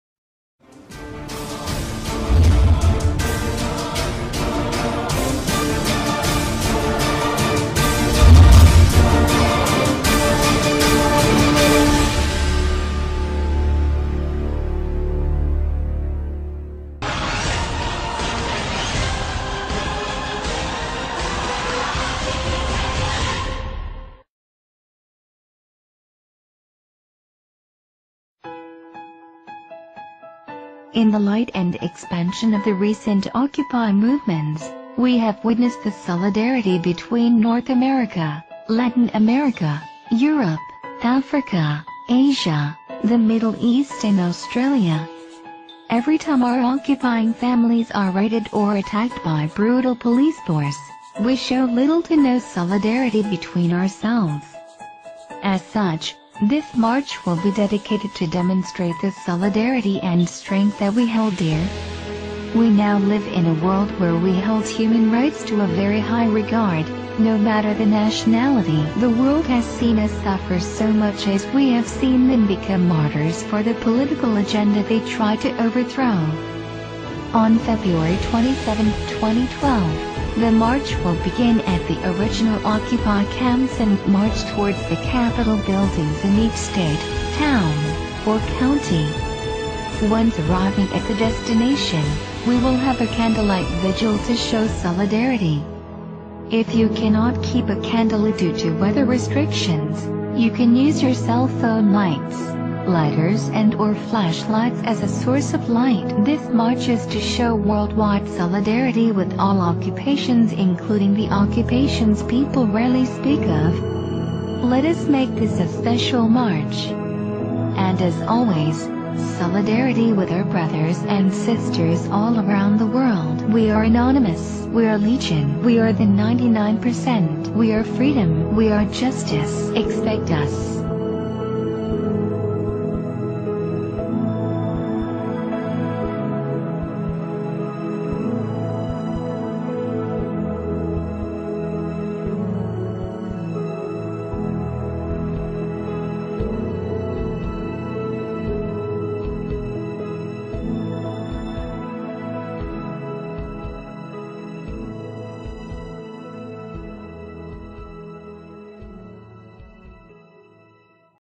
Anonymous: Occupy Solidarity March 2/27/2012
TO ALL OCCUPY MOVEMENTS! Please spread this message far and wide! Worldwide Day of Solidarity to be planned on February 27th. We will all join together in unity, from OccupyWallStreet and OccupyOakland to OccupyNigeria and OccupyAthens.

In the light and expansion of the recent Occupy movements, we have witnessed the solidarity between North America, Latin America, Europe, Africa, Asia, the Middle East and Australia. Every time our Occupying families are raided or attacked by brutal police force, we show little to no solidarity between ourselves. As such, this march will be dedicated to demonstrate the solidarity and strength that we hold dear. We now live in a world where we hold human rights to a very high regard, no matter the nationality.

The world has seen us suffer so much as we have seen them become martyrs for the political agenda they try to overthrow. On February 27th, 2012, the march will begin at the original Occupy camps and march towards the Capitol buildings in each state, town, or county. Once arriving at the destination, we will have a candlelight vigil to show solidarity. If you cannot keep a candle lit due to weather restrictions, you can use your cell phone lights, lighters and/or flash lights as a source of light.

This march is to show worldwide solidarity with all Occupations including the Occupations people rarely speak of. Let us make this a special march. And as always, solidarity with our brothers and sisters all around the world.

We are The 99 percent.
We are freedom.
We are justice.
Expect us.